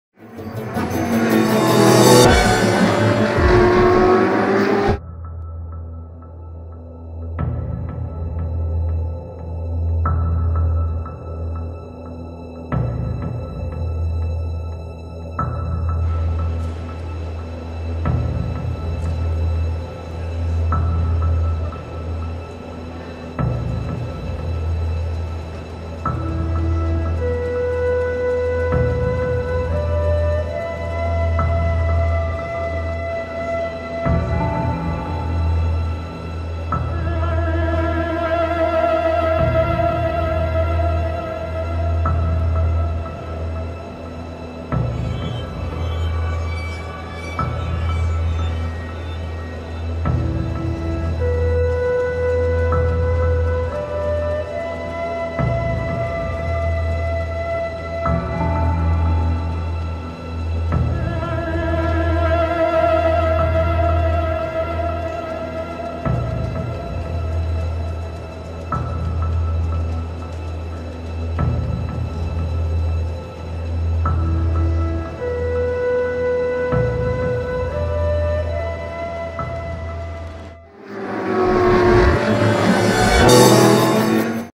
Nissan Concept Cars at Nissan 360
Nissan Concept Cars at Nissan 360: Nissan XTreme, Nissan Friend-ME, Nissan Resonance.